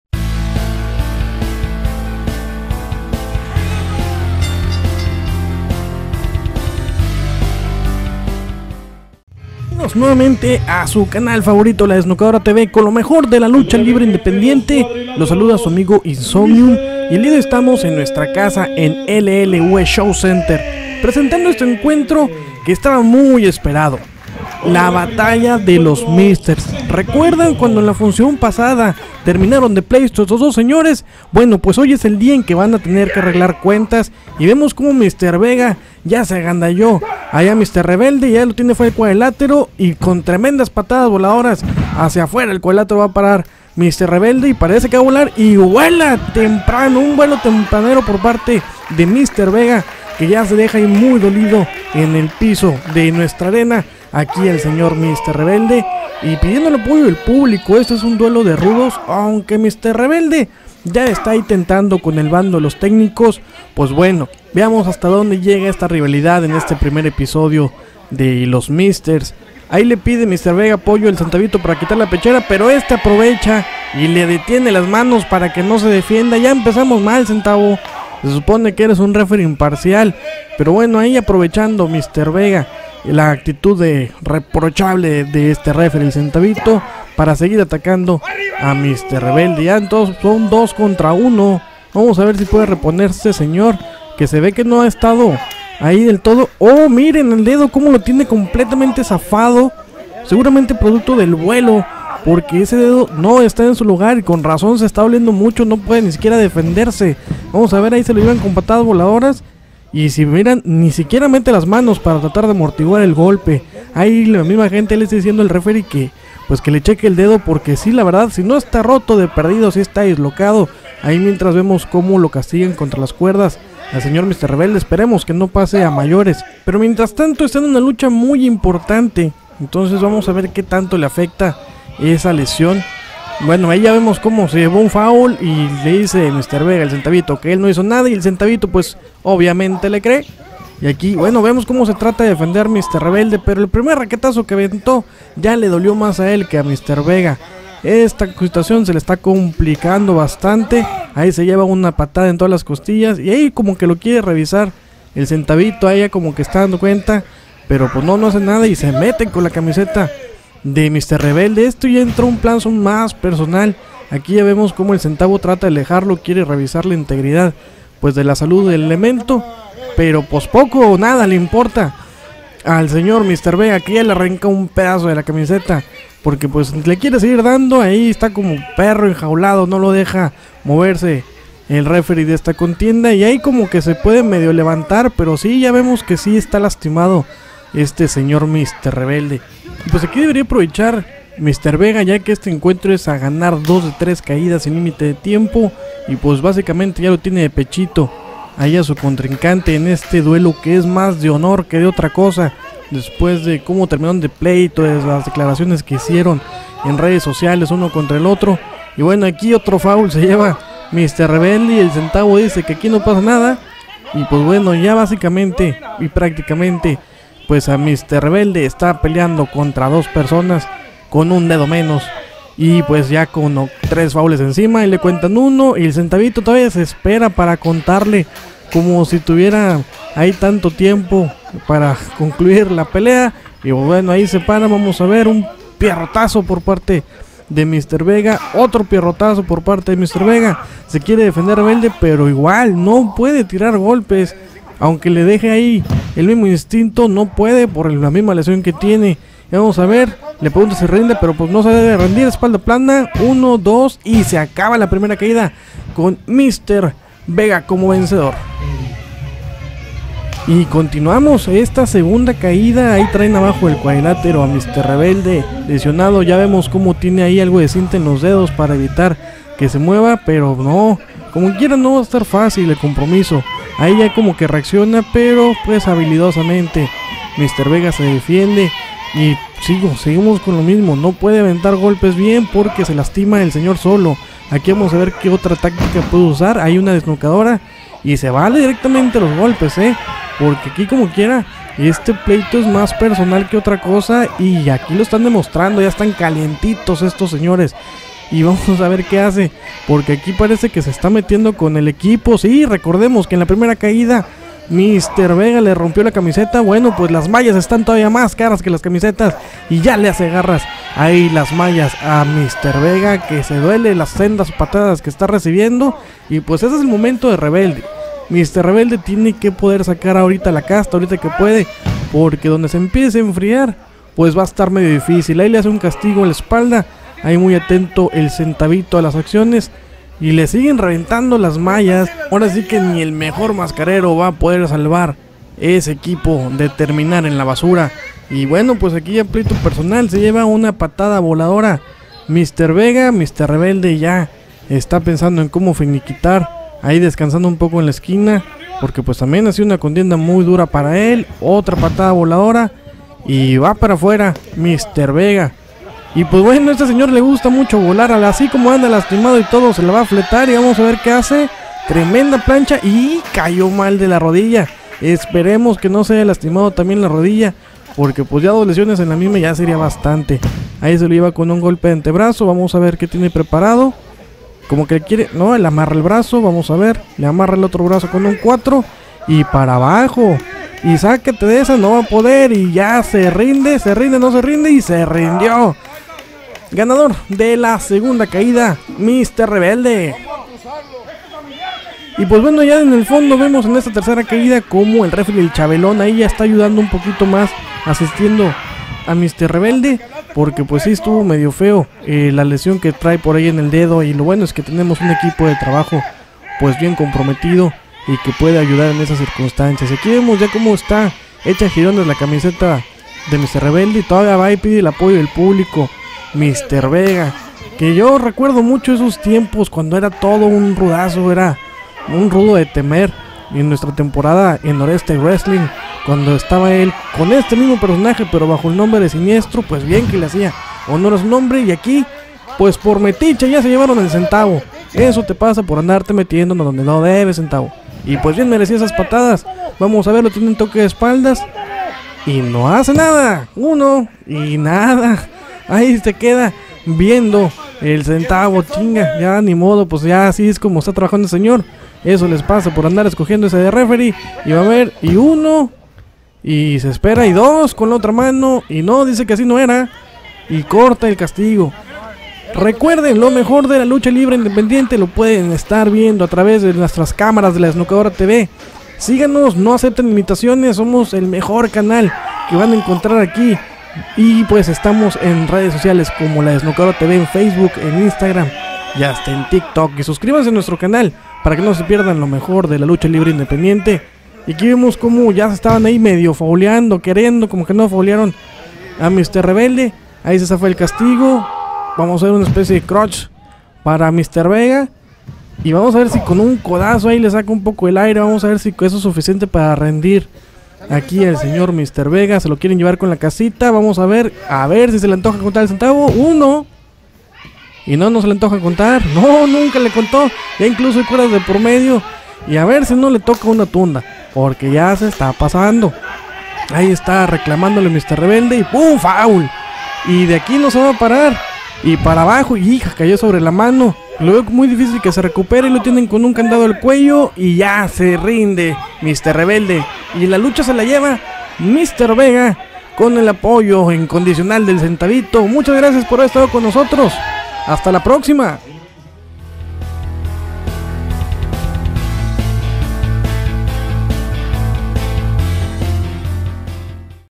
Mr Vega VS Mr Rebelde @ Lucha Libre Universtal Entertainment Show Center
La que pudiera haber sido una de las mejores parejas del año, Mr Vega y Mr Rebelde acabaron en malos términos en la función de aniversario de LL UE, y se enfrentaron en un mano a mano donde tristemente Mr Rebelde terminó lesionado.